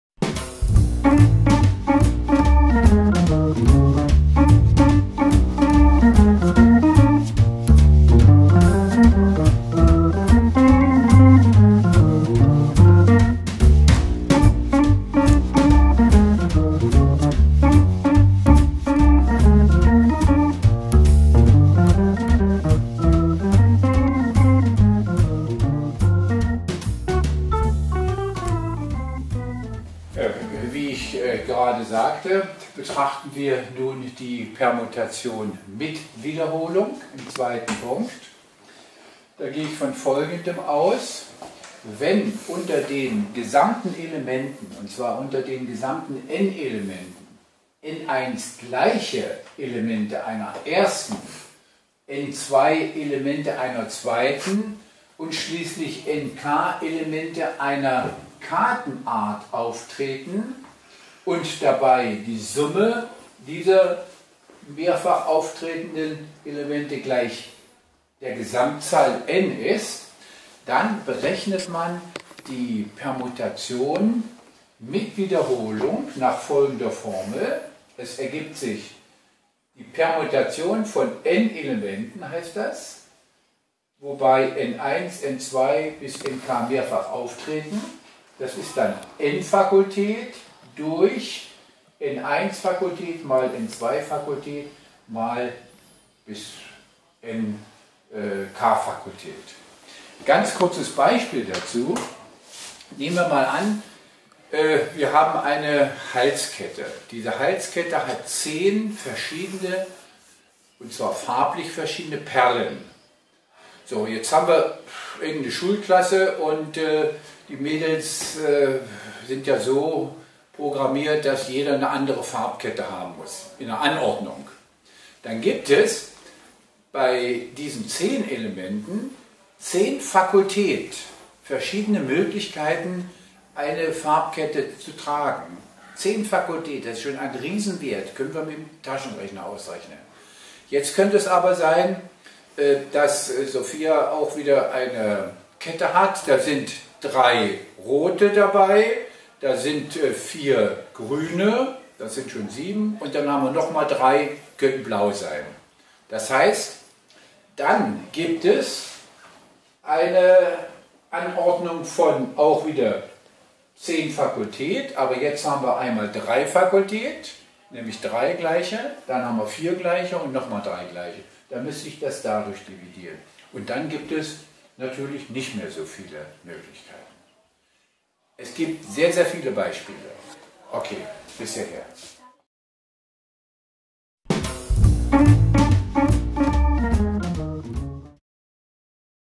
Kombinatorik - Permutationen mit Wiederholung
Die Berechnung der Permutation mit Wiederholung wird gezeigt.